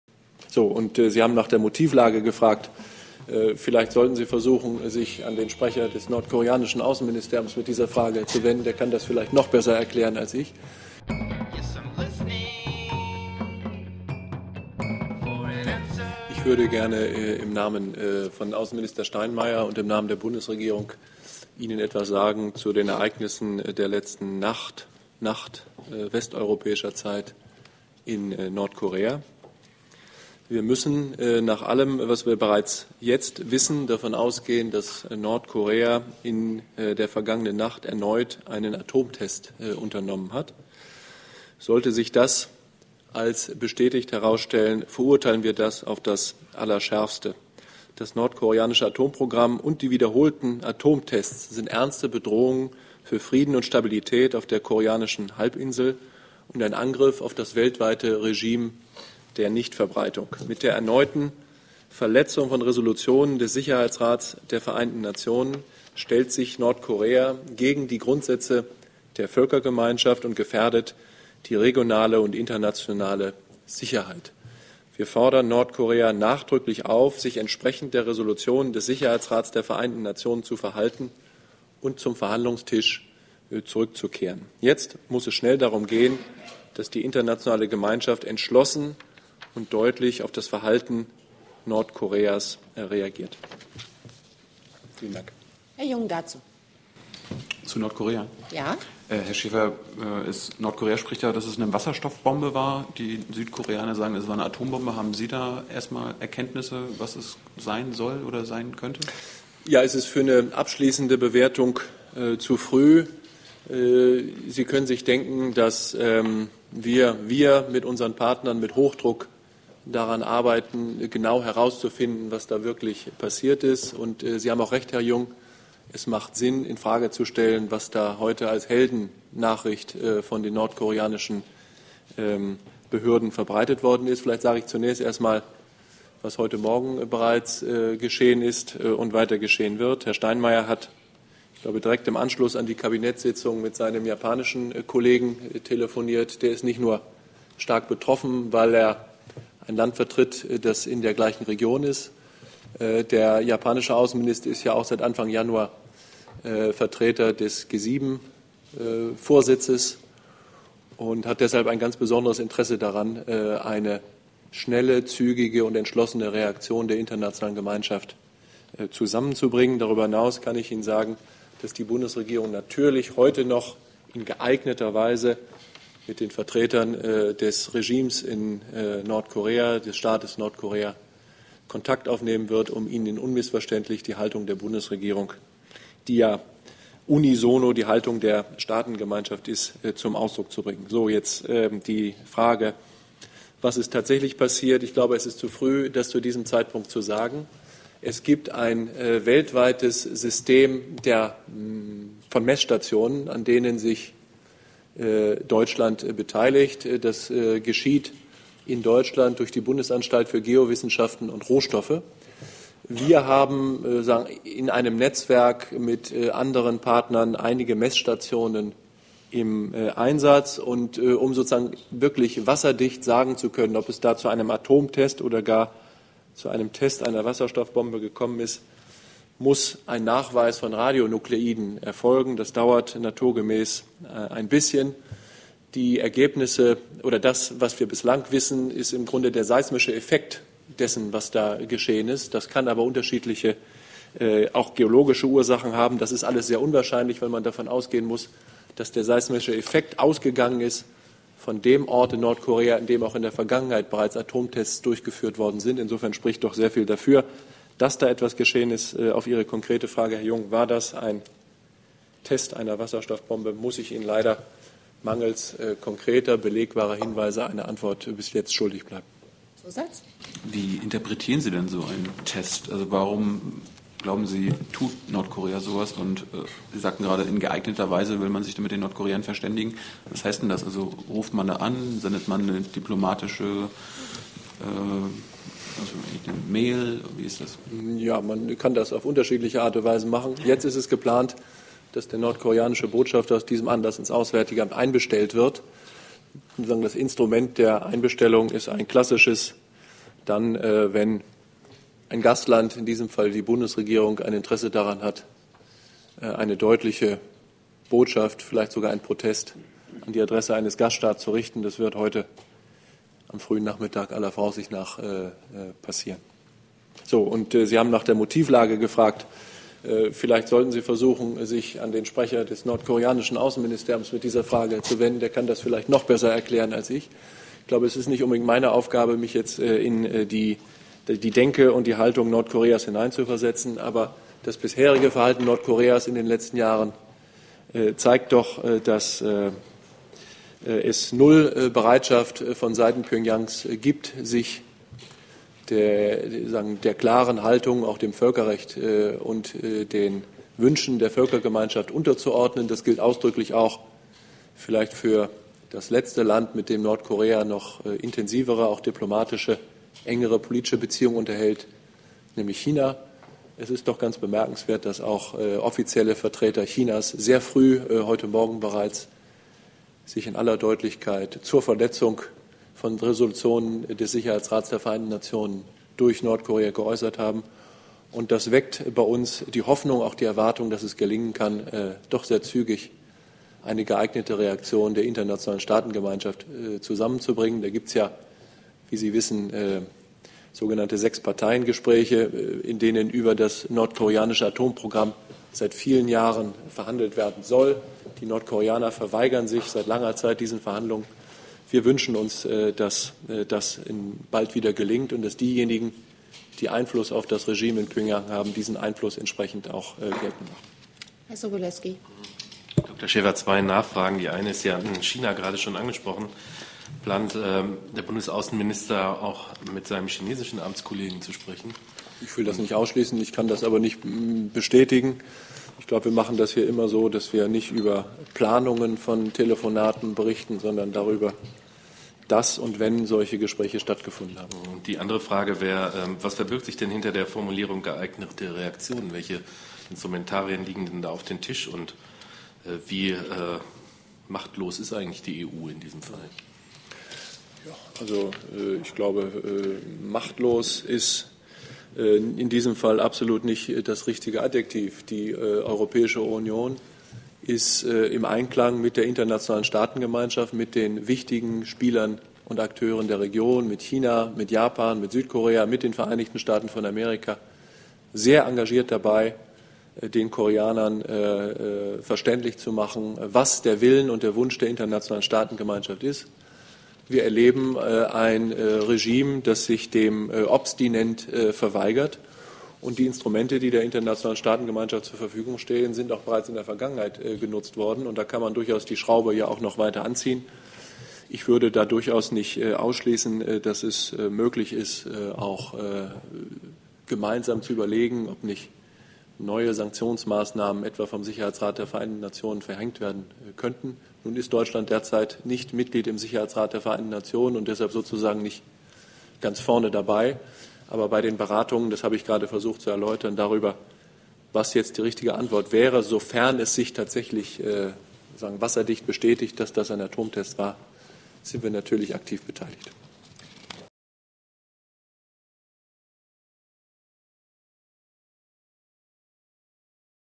Nordkorea testet Atombombe: Die Bundesregierung reagiert
Nordkorea hat gestern Nacht eine Atombombe getestet:
Die Bundesregierung "verurteilt das aufs Allerschärfste" und bestellt den Botschafter ein. Wir wollten daraufhin vom Auswärtiges Amt wissen, ob es denn wirklich eine Wasserstoffbombe war oder nicht doch "nur" eine Atombombe. Wie interpretiert die Bundesregierung diesen Test, warum tut Nordkorea sowas? Und wie kann man sich die Kommunikation mit dem nordkoreanischen Regime nun vorstellen?

Bitte unterstützt unsere Arbeit finanziell:
Tilo Jung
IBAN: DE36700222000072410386
BIC: FDDODEMMXXX
Verwendungszweck: BPK